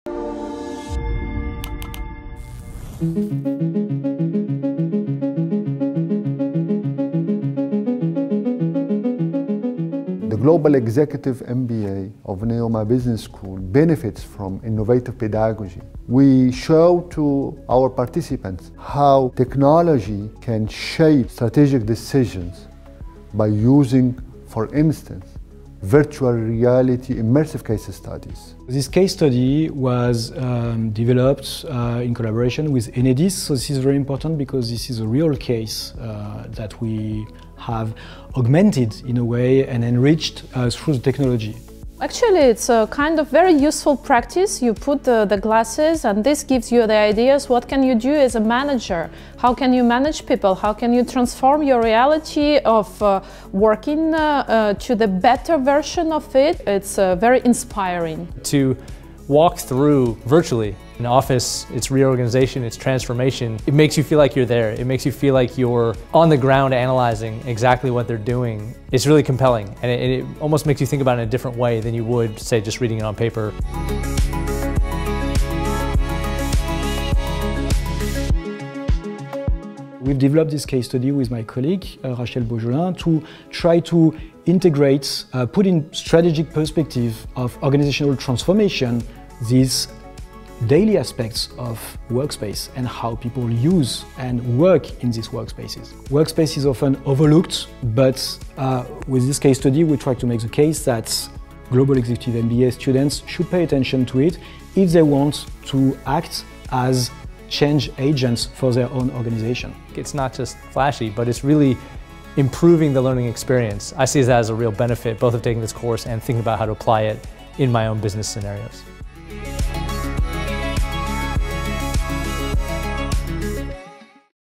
NEOMA's Global Executive MBA : an innovative pedagogy
This is why in NEOMA's Global Executive MBA, we integrate innovation directly into the curriculum. Virtual Reality allows participants to immerse themselves in real business situations and understand how technology can help make strategic decisions.